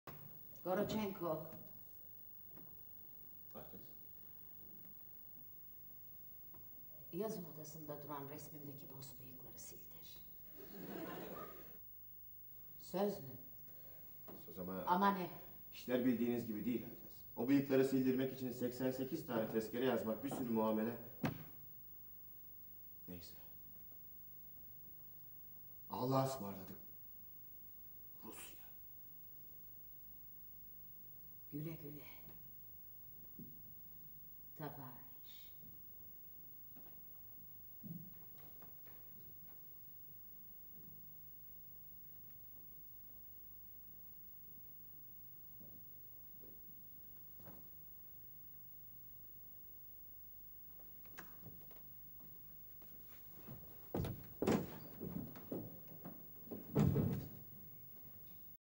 ŞAHANE ZÜĞÜRTLER/ Nevra Serezli- Hüseyin Köroğlu
ŞAHANE ZÜĞÜRTLER (Tovarich)... 1991... DORMEN TİYATROSU... Metin Serezli ustanın yönettiği; Nevra Serezli ustayla sahneyi paylaştığım çok kıymetli anlar... Metin Serezli ustaya selam olsun...🌹